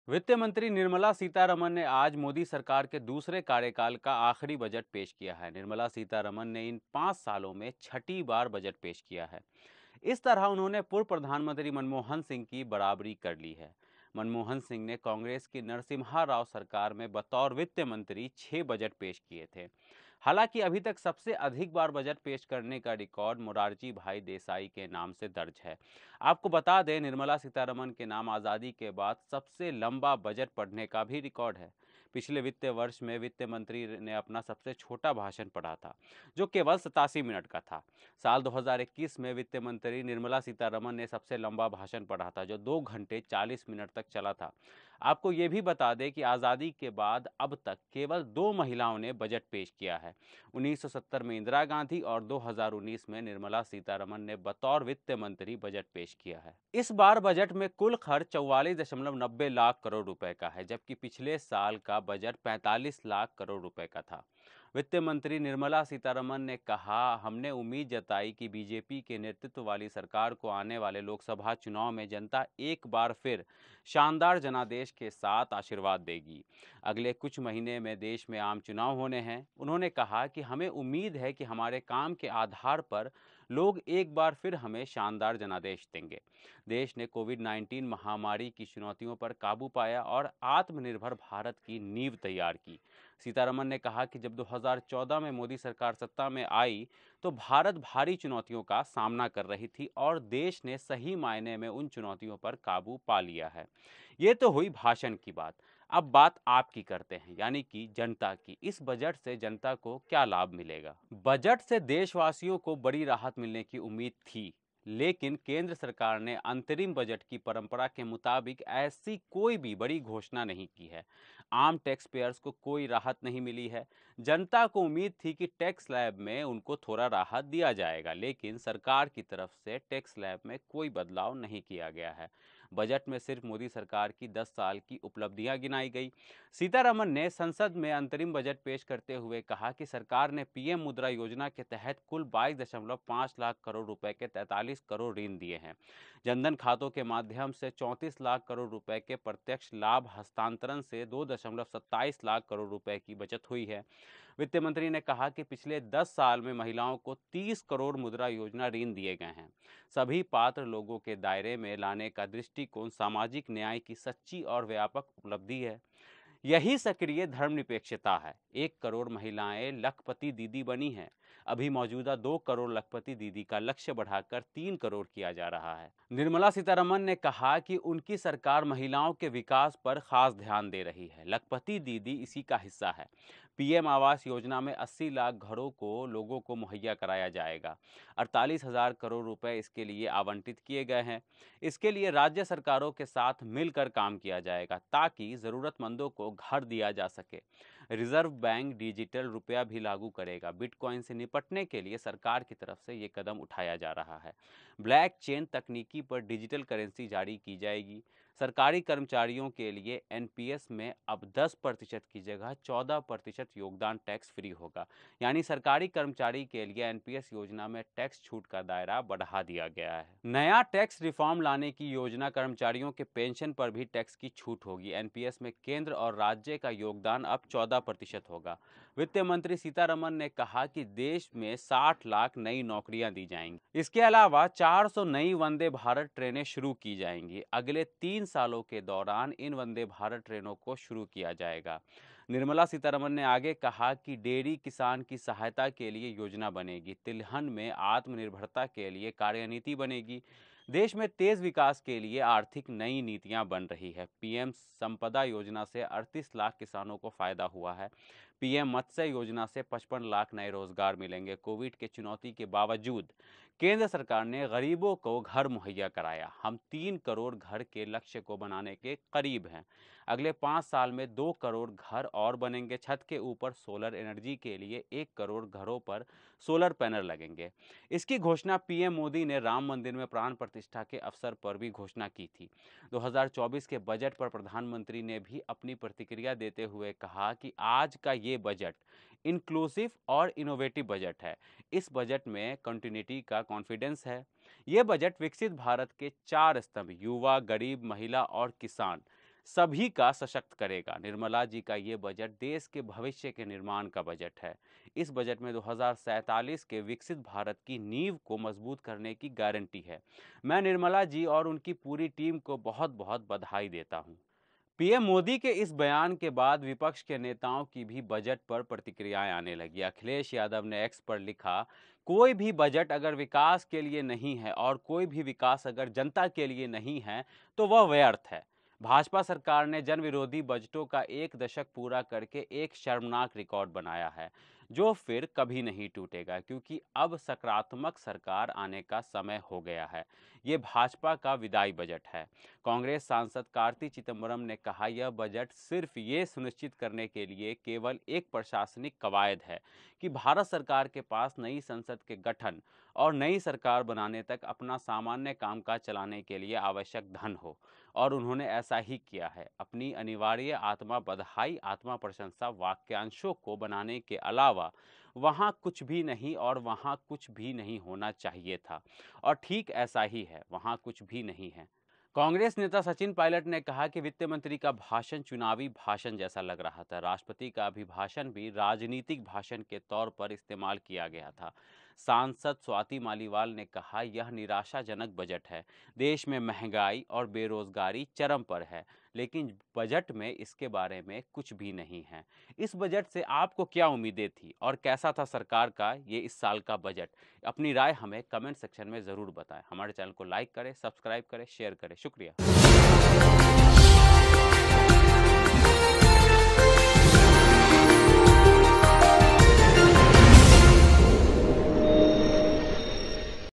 बजट 2024 से मिडिल क्लास की जेब पर कितना असर पड़ा? | Union Budget 2024 | PM Modi | Middle Class
बजट से देशवासियों को बड़ी राहत मिलने की उम्मीदें थीं।  लेकिन केंद्र सरकार ने अंतरिम बजट की परंपरा के मुताबिक ऐसी कोई बड़ी घोषणा नहीं की है। आम टैक्स पेयर्स को कोई राहत नहीं मिली है।जनता को उम्मीद थी कि टैक्स स्लैब में उनको थोड़ी राहत दी जाएगी लेकिन सरकार की तरफ से टैक्स स्लैब में कोई बदलाव नहीं किया गया है बजट में सिर्फ मोदी सरकार की दस साल की उपलब्धियां गिनाई गयी है। सीतारमण ने संसद में अंतरिम बजट पेश करते हुए कहा कि सरकार ने पीएम मुद्रा योजना के तहत कुल 22.5 लाख करोड़ रुपये के 43 करोड़ ऋण दिए हैं। जनधन खातों के माध्यम से 34 लाख करोड़ रुपये के प्रत्यक्ष लाभ हस्तांतरण से 2.7 लाख करोड़ रुपये की बचत हुई है। वित्त मंत्री ने कहा कि पिछले 10 साल में महिलाओं को 30 करोड़ मुद्रा योजना ऋण दिए गए हैं। सभी पात्र लोगों को दायरे में लाने का दृष्टिकोण सामाजिक न्याय की सच्ची और व्यापक उपलब्धि है और यही सक्रिय धर्मनिरपेक्षता है। एक करोड़ महिलाएं लखपति दीदी बनी हैं। अभी मौजूदा 2 करोड़ लखपति दीदी का लक्ष्य बढ़ाकर 3 करोड़ किया जा रहा है। वित्त मंत्री ने कहा कि उनकी सरकार महिलाओं के विकास पर खास ध्यान दे रही है। लखपति दीदी इसी का हिस्सा हैं।पीएम आवास योजना में 80 लाख घरों को लोगों को मुहैया कराया जाएगा। 48 हजार करोड़ रुपये इसके लिए आवंटित किए गए हैं। इसके लिए राज्य सरकारों के साथ मिलकर काम किया जाएगा। ताकि जरूरतमंदों को घर दिया जा सके।रिजर्व बैंक डिजिटल रुपया भी  लागू करेगा। बिटकॉइन से निपटने के लिए सरकार की तरफ से ये कदम उठाया जा रहा है । ब्लैक चेन तकनीक पर डिजिटल करेंसी जारी की जाएगी।सरकारी कर्मचारियों के लिए एनपीएस में अब 10% की जगह 14% योगदान टैक्स फ्री होगा। यानी सरकारी कर्मचारियों के लिए NPS योजना में टैक्स छूट का दायरा बढ़ा दिया गया है । नया टैक्स रिफॉर्म लाने की योजना। कर्मचारियों के पेंशन पर भी टैक्स की छूट होगी । NPS में केंद्र और राज्य का योगदान अब 14% होगा।वित्त मंत्री सीतारमण ने कहा कि देश में 60 लाख नई नौकरियां दी जाएंगी। इसले अलावा 400 नई वंदे भारत ट्रेनें शुरू की जाएंगी। अगले 3 सालों के दौरान इन वंदे भारत ट्रेनों को शुरू किया जाएगा।